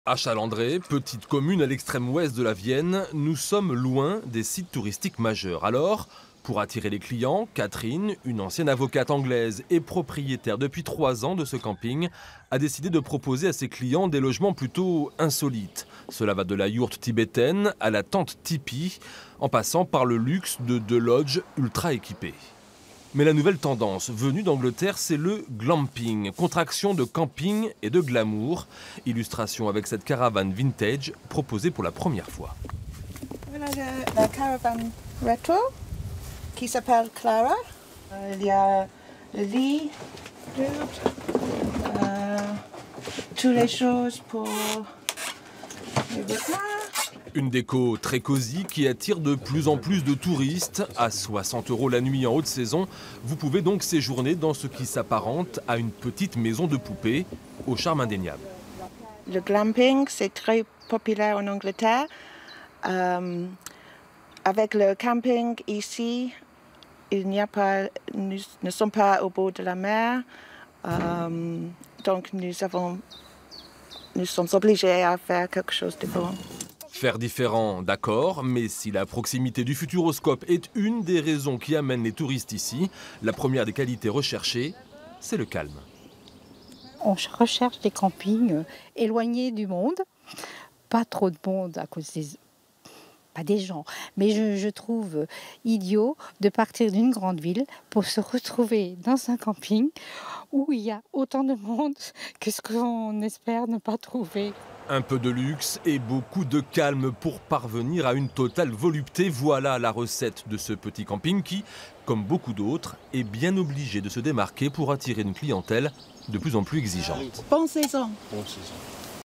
Le glamping à Chalandray dans la Vienne
Connaissez-vous le glamping ? C'est quand le camping se metau glamour. C'est la recette choisie par la propriétaire du camping de Chalandray dans la Vienne.
Reportage de Jérôme Vilain, Laurent Gautier et Josiane Etienne